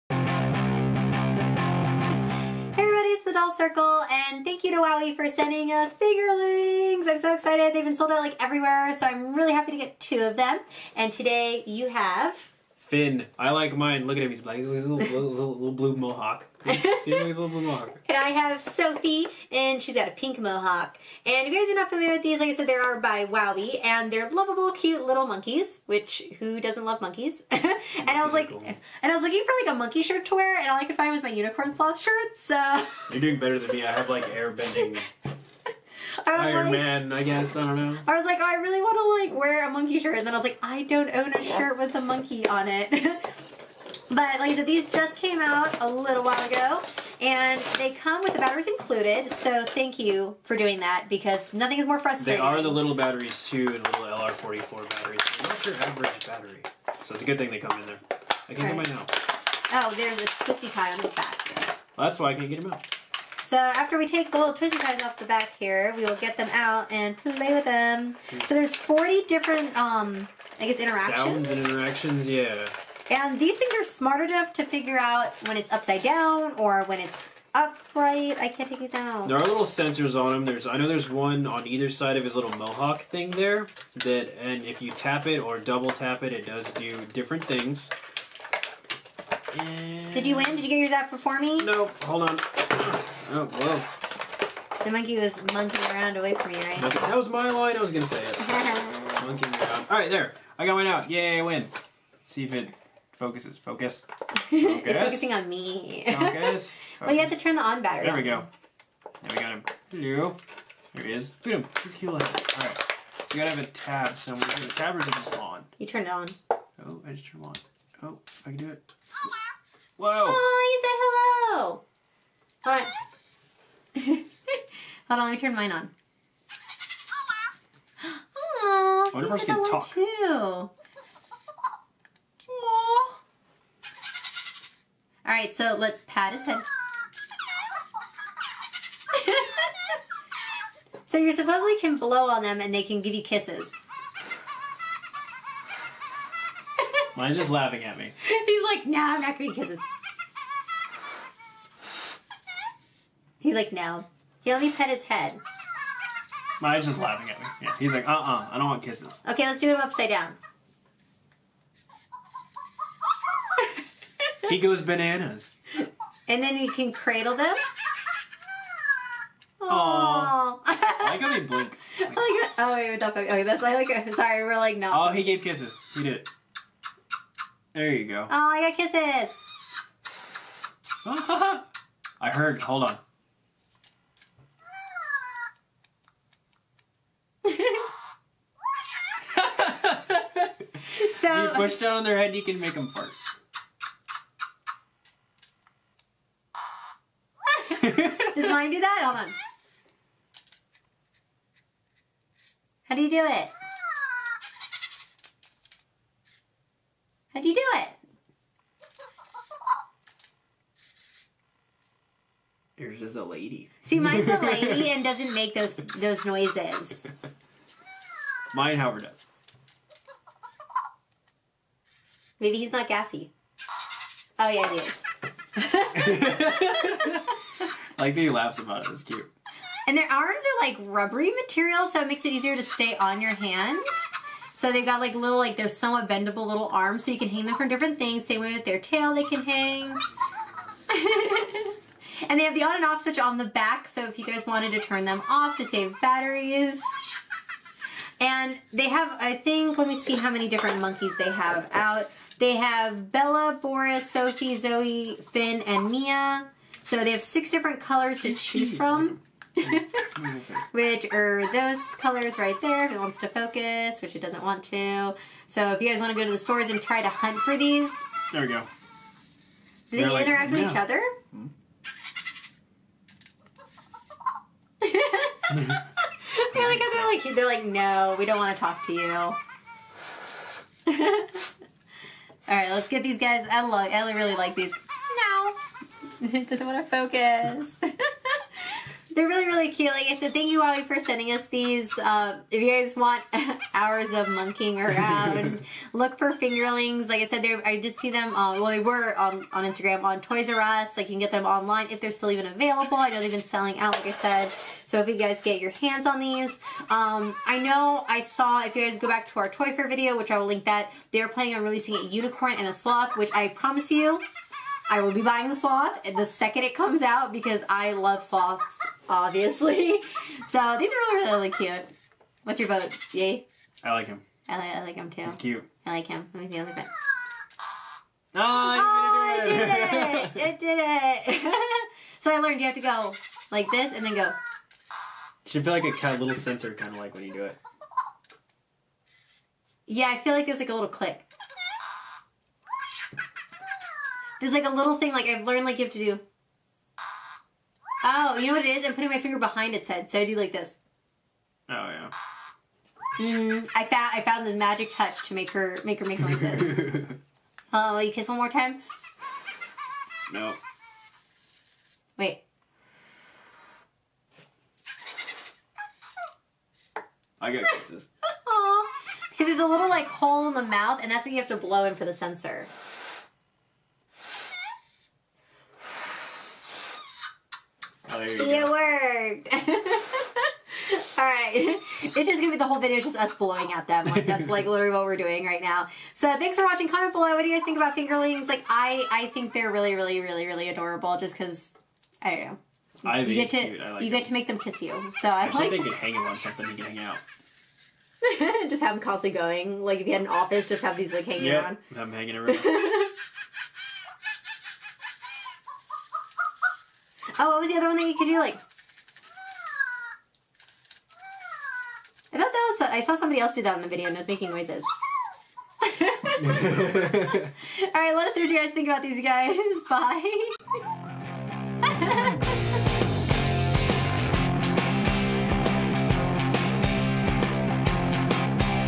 FINGERLINGS! Unboxing New Fun Monkey Toys by Wowwee
Thank you to Wowwee for sending us these fun toys!